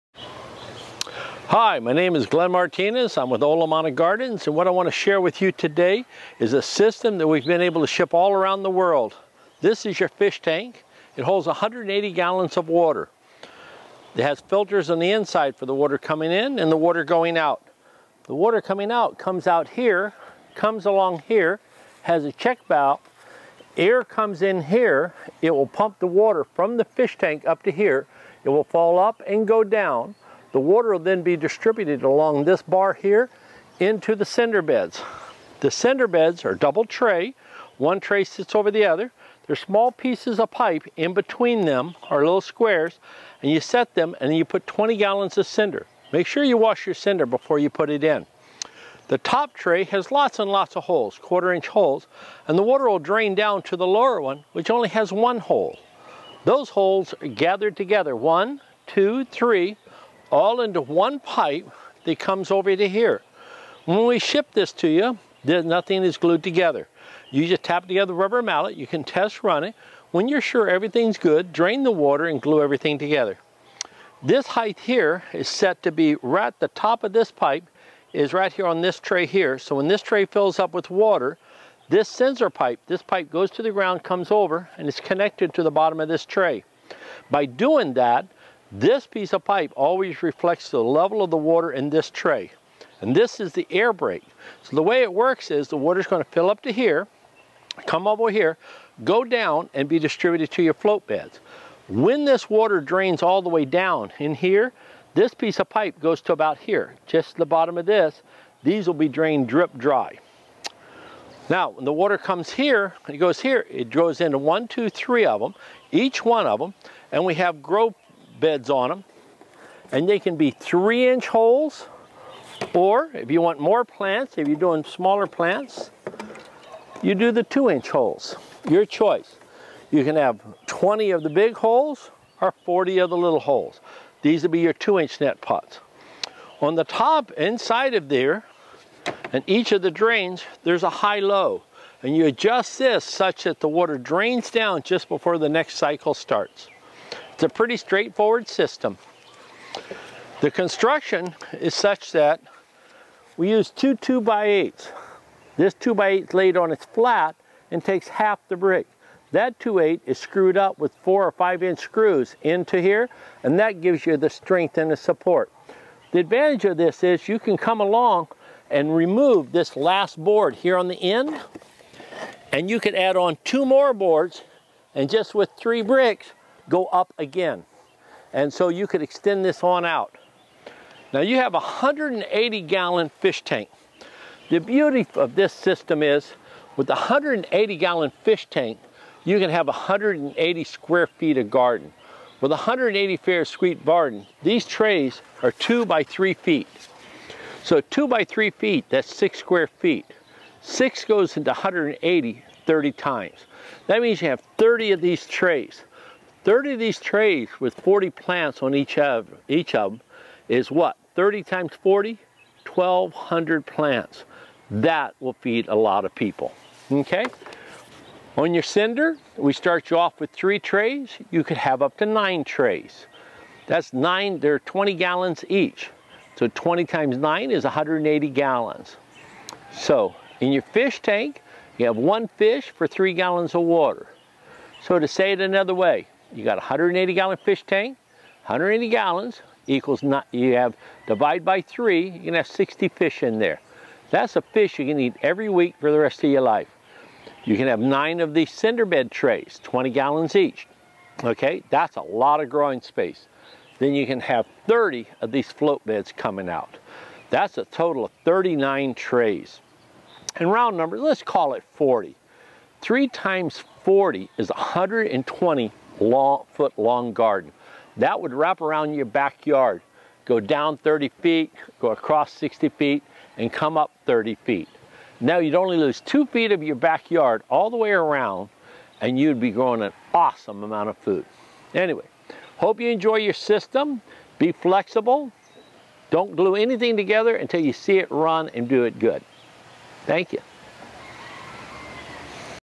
Big Island Aquaponics for Wendy
Glenn Martinez of Olomana Gardens has created yet another aquaponics triple, double cinder beds that gets it water from the 180 gallon fish tank pushed by using an air pump feature that moves the water through and up into the bio-filter. The  bio-filter cinder beds will breathe (inhale and exhale) through the cinders by the ebb and flow from the siphon that controls how much water will fill up in the cinder bed before the flush. Then the water will get released into the deep trough beds, which the plants will grow in the water.
  The water then returns from the float beds back to the fish tank all nice and clean. Amazing stuff and good vegetable garden growing.
  It's all about organic, natural food source. Try it, taste it, and see why you should grow your own food.